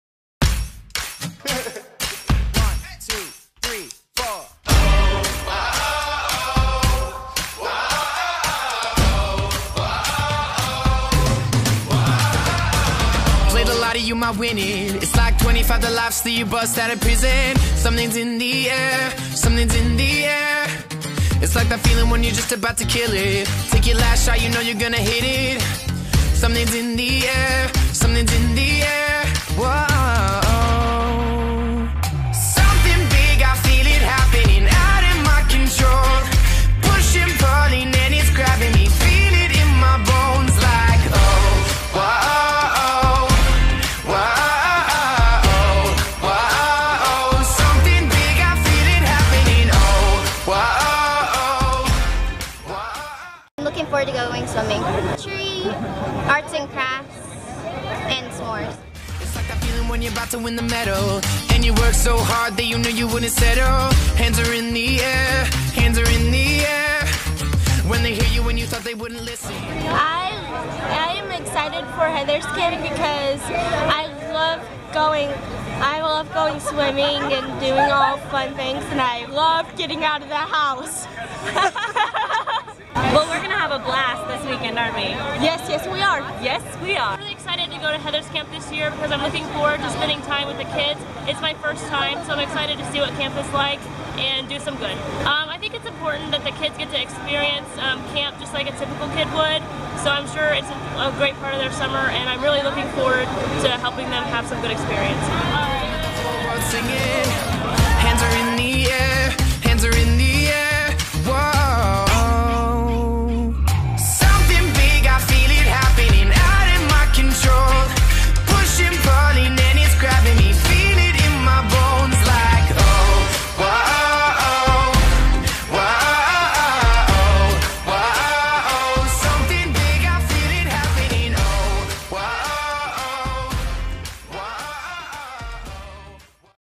2016 Heather's Camp ... getting on the buses
Do you remember your first bus trip to summer camp? Nearly 100 youth who are blind or visually impaired are creating memories at Heather's Camp Inc., a place that gives them the same outdoor experiences their typically sighted peers receive. Watch the videod and think about this ...If they were this excited before they even boarded the buses, can you imagine have much fun they have at camp?

Envision is proud to be the presenting sponsor of Heather's Camp. Thank you to Wichita State Delta Gamma for your support!